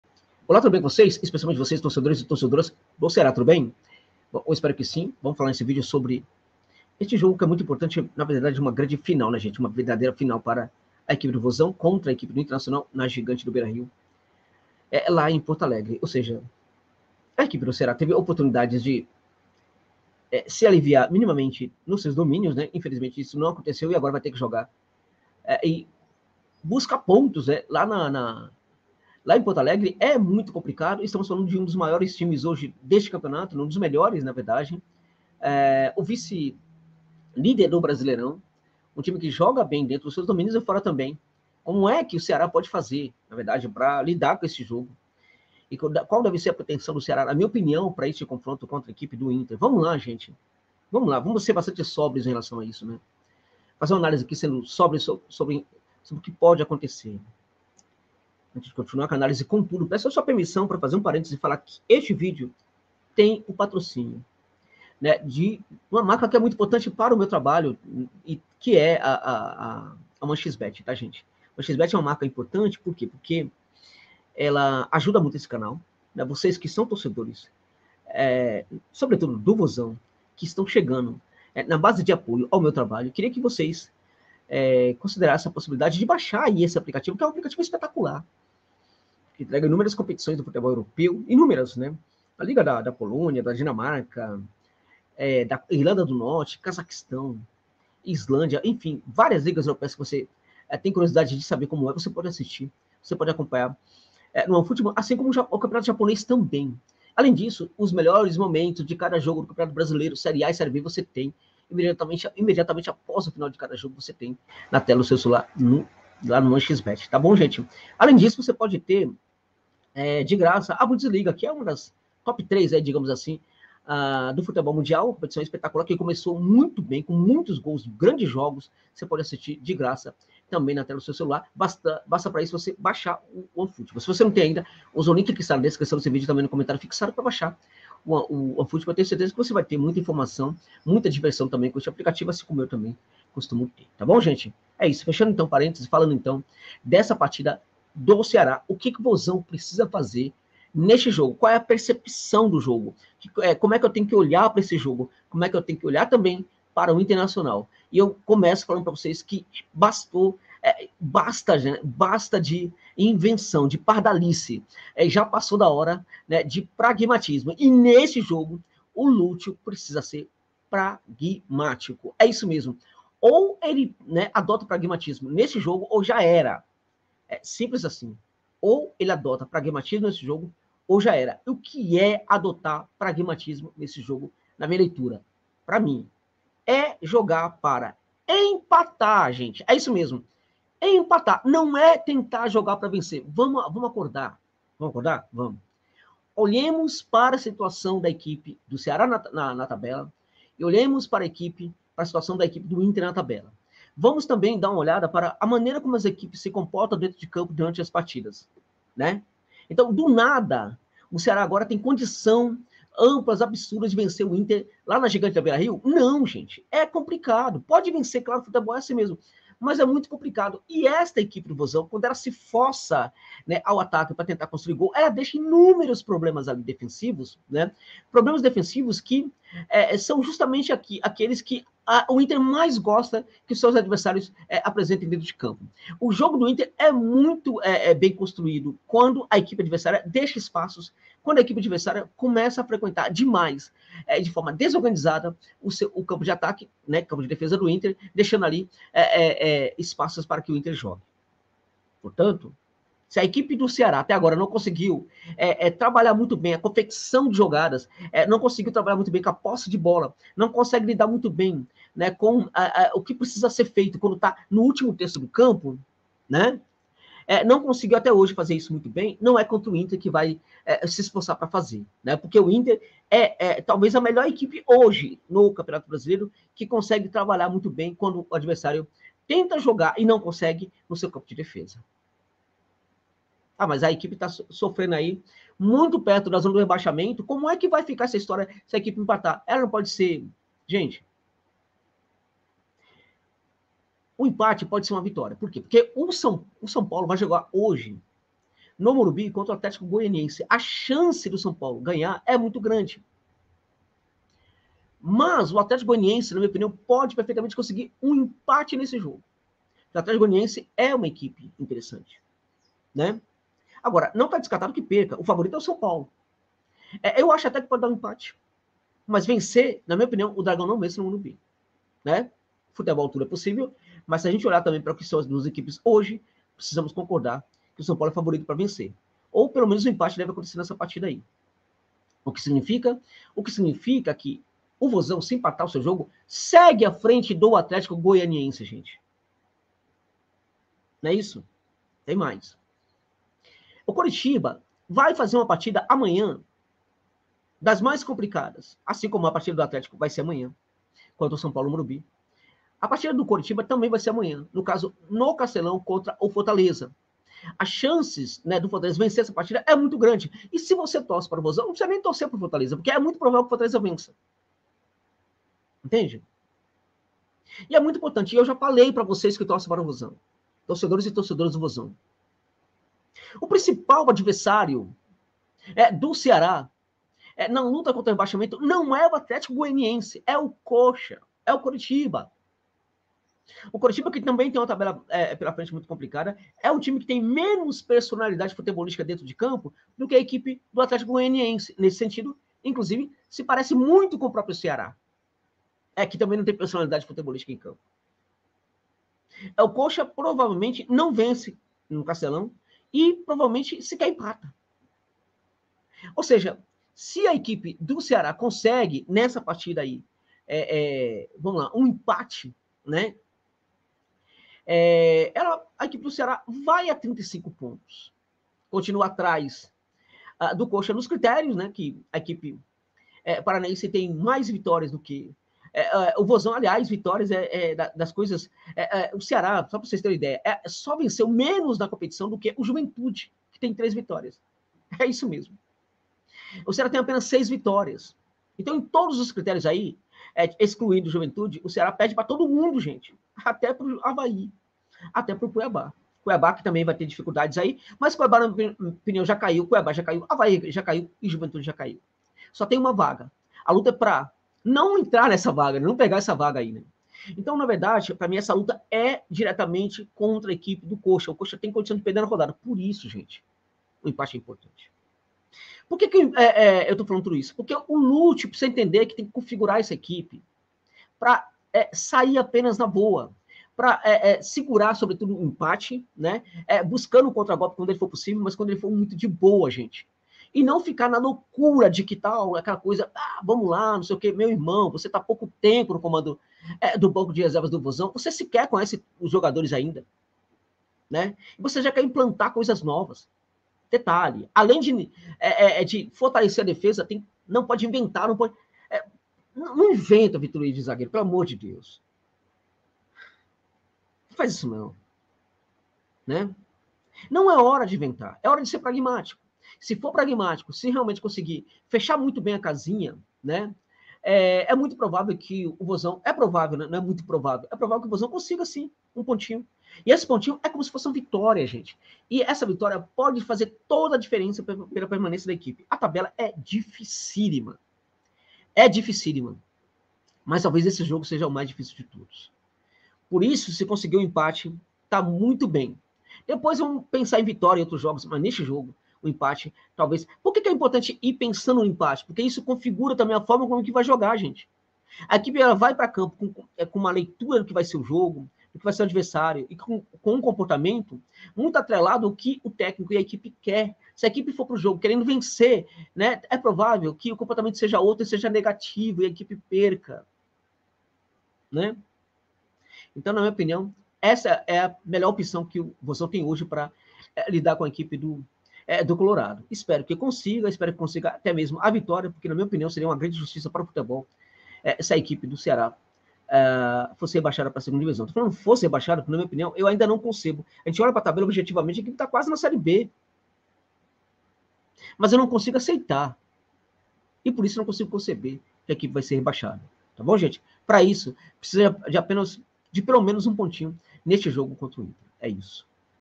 Jogo contra o Inter para o Ceará sobreviver na Série A
Neste vídeo, temos uma análise sobre a situação do Ceará Esporte Clube, permeada pelas chances que o Vozão tem de se dar bem na rodada. Para tanto, a equipe nordestina não pode perder essa partida de forma alguma. Contudo, precisa necessariamente vencer? Por que a percepção desse jogo e também da própria rodada podem fazer toda a diferença? E por que o Ceará precisa se preocupar com duas outras equipes na rodada? Tudo isso e muito mais se analisa neste vídeo.